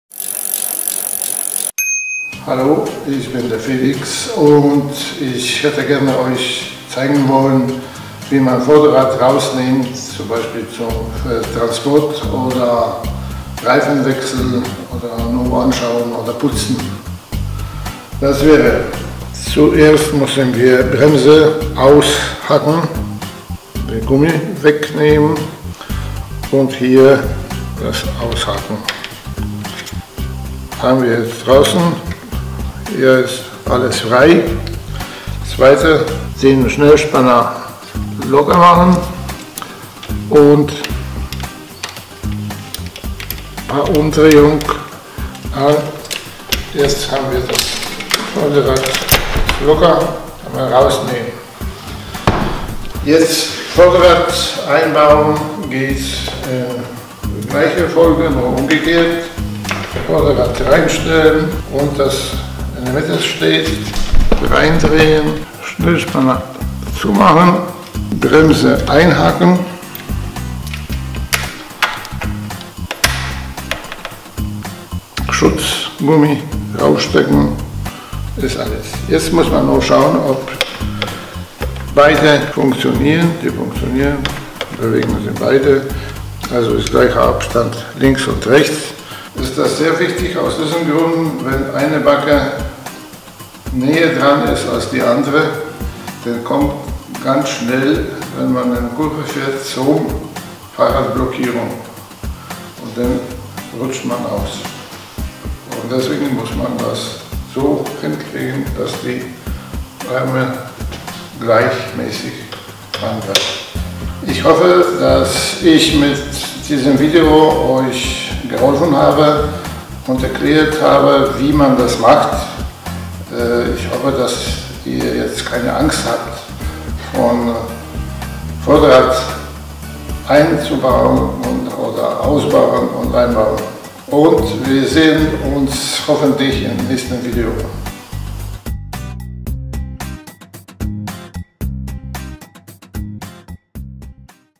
Fahrrad Vorderrad Ein- & Ausbau V-Bremse - Fehler vermeiden - Fahrrad.org
In diesem Video zeigen wir euch, wie ihr ganz einfach das Vorderrad bei einem Fahrrad mit V-Bremse ausbauen könnt. Viel Spaß!